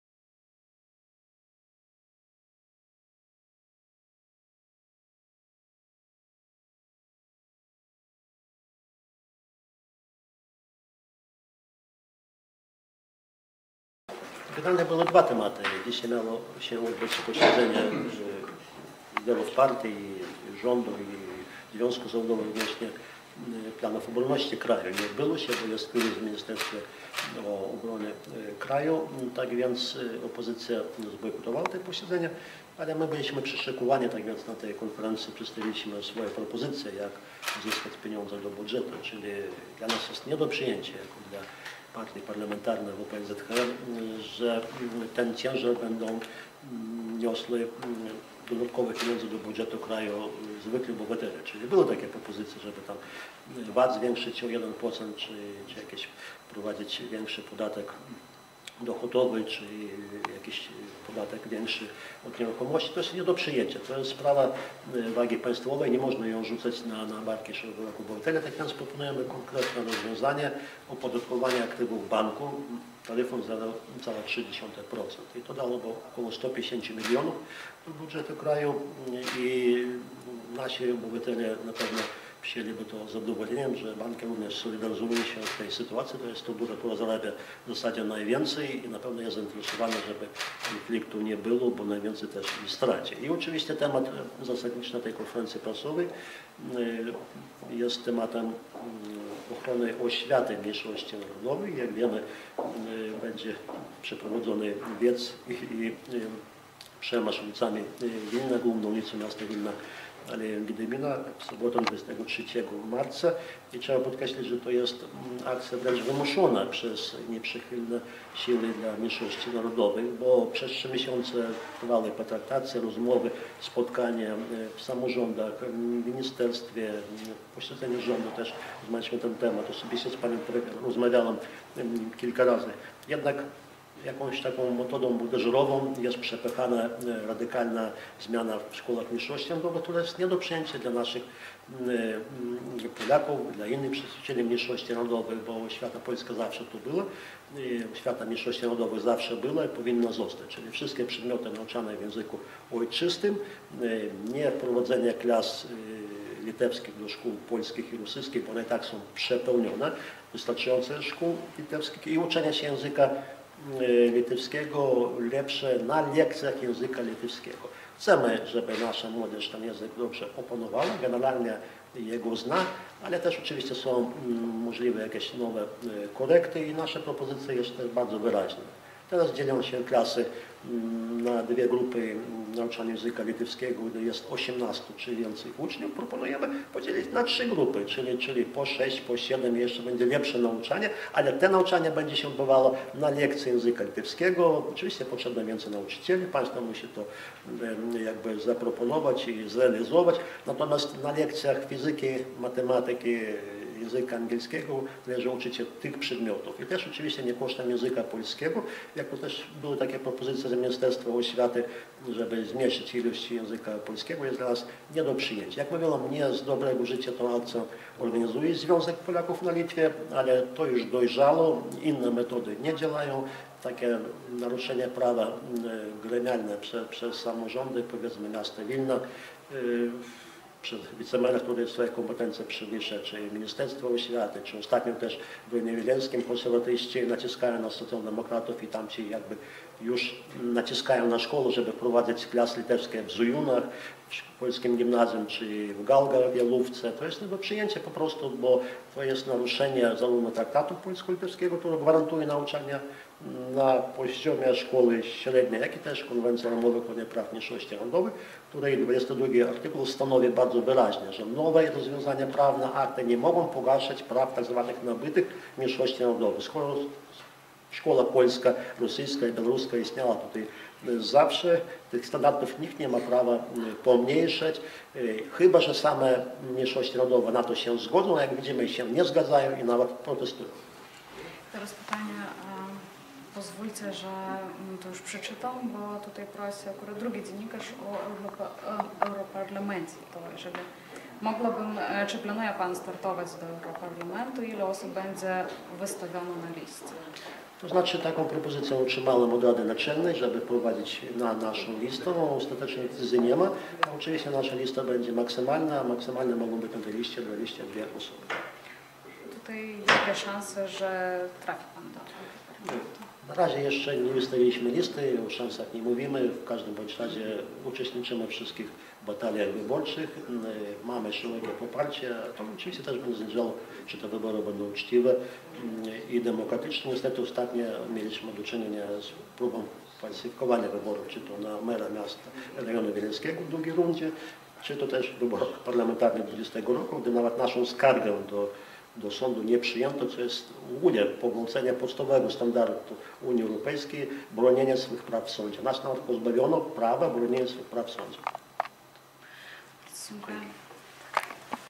После пресс-конференции в Сейме Литвы (на польском языке)
В понедельник, 18 марта, в Сейме Литвы прошла пресс-конференция депутатов парламента Чеслава Ольшевского и Риты Тамашунене «О политическом соглашении по обороне и дискриминации школ национальных меньшинств». В ее работе приняли также участие председатель Избирательной акции поляков Литвы-Союза христианских семей (ИАПЛ-СХС), европарламентарий Вальдемар Томашевский и депутат совета Вильнюсского городского самоуправления Эдита Тамошюнайте